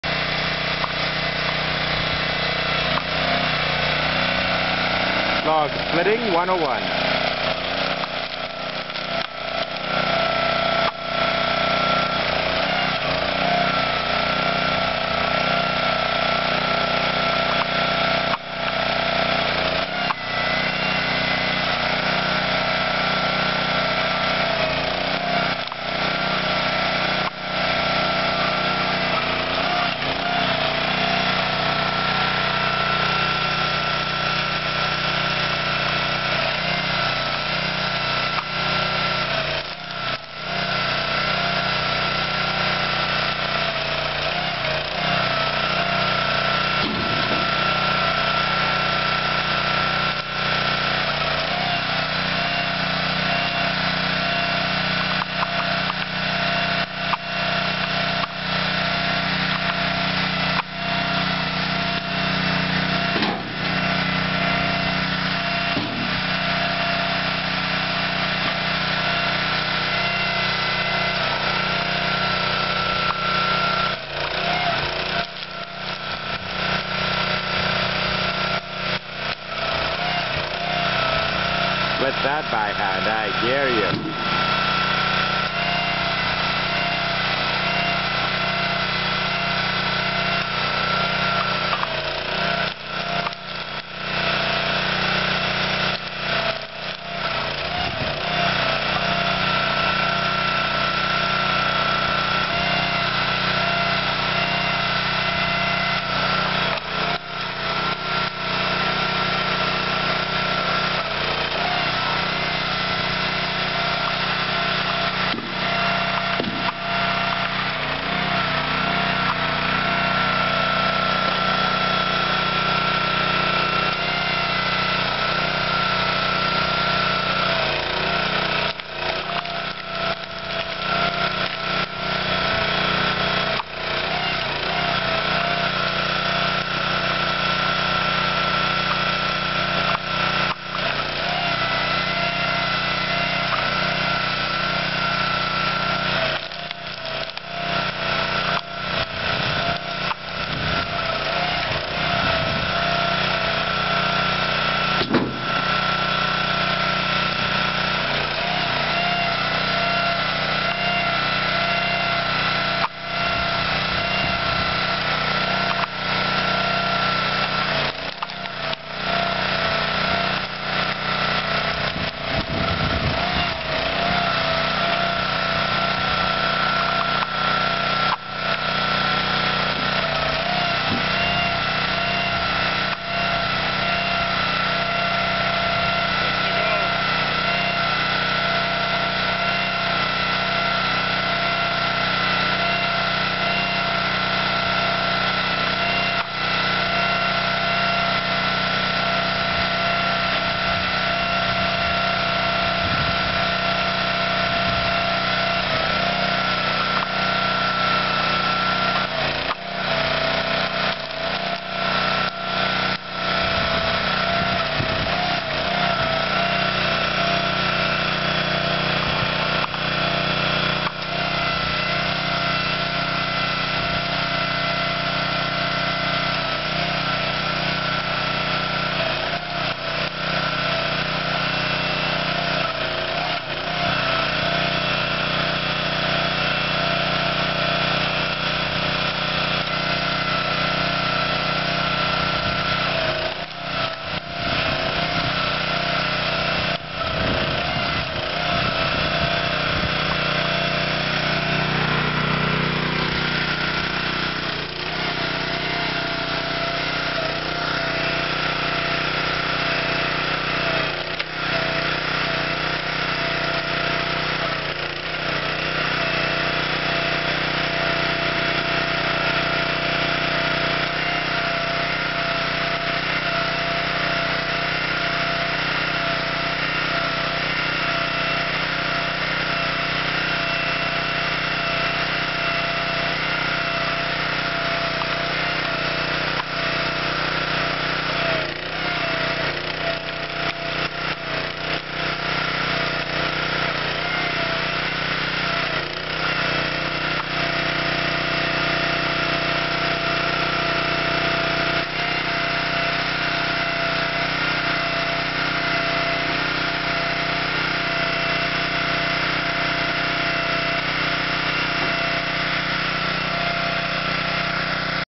Log Splitting 101
Splitting a log with the aide of  a log splitter